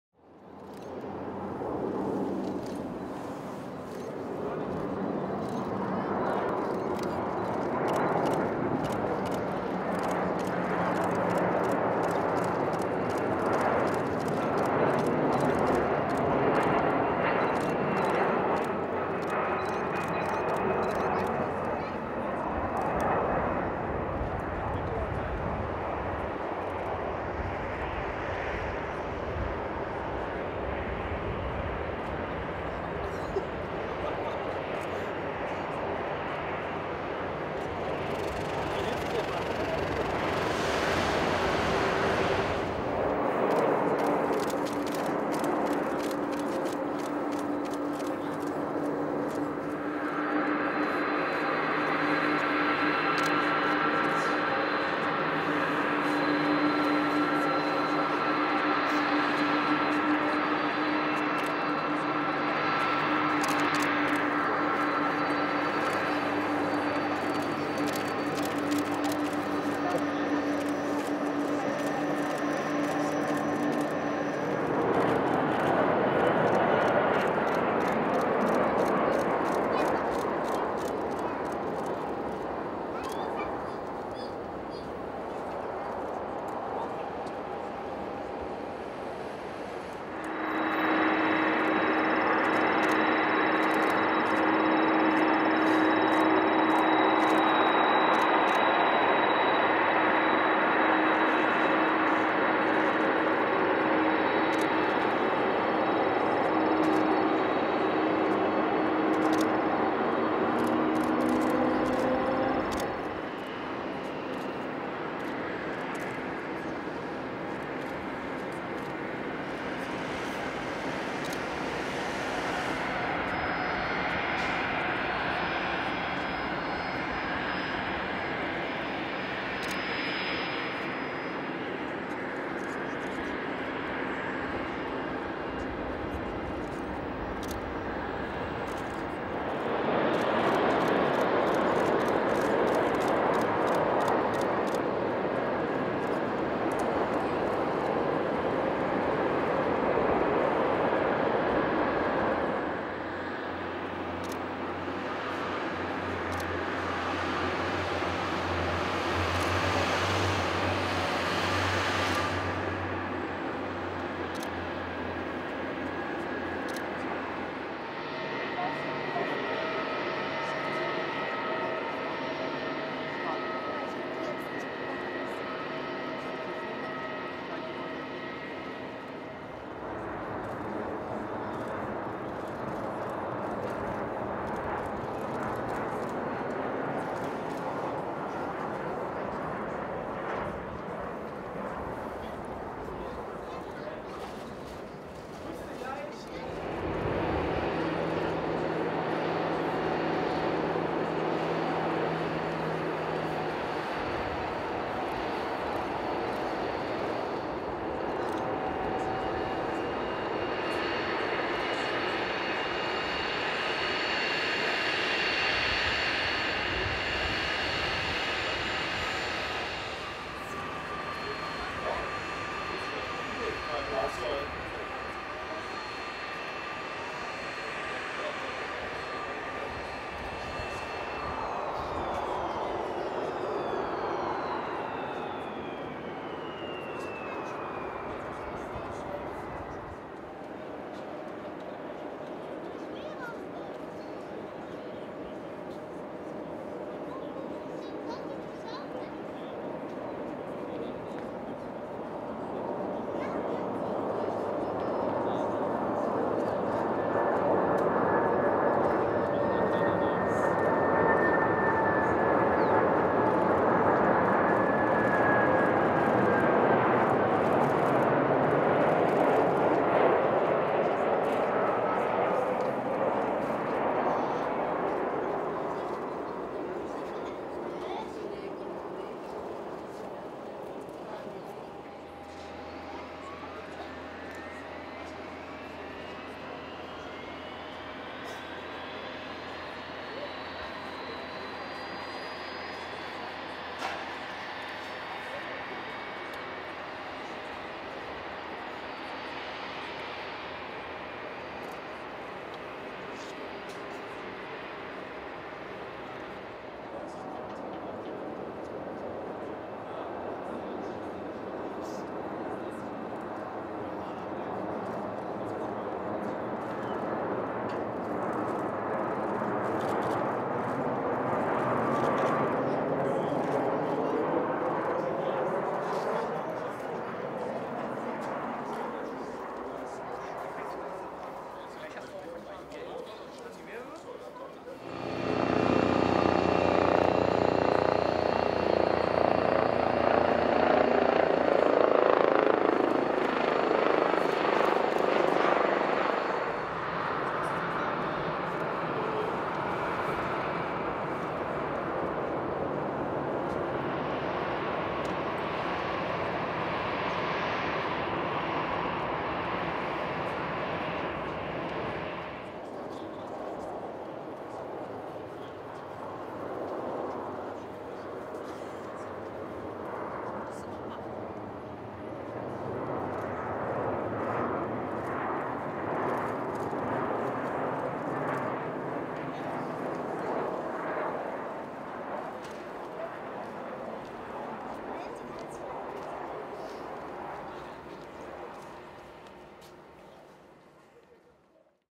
Planespotting CGN - Köln/Bonn Airport - 07.04.2013
Recorded with SONY HXR-NX70E.

Nach den Erfahrungen des letzten CGN-Spottings habe ich diesmal einige Verbesserungen umgesetzt: Die Windgeräusche per Low-Cut-Filter ausgeblendet, die Helligkeit reduziert und Fokus weitgehend manuell bedient, da der Autofokus mit den starken Sonnenreflektionen auf den Fliegern oft überfordert ist.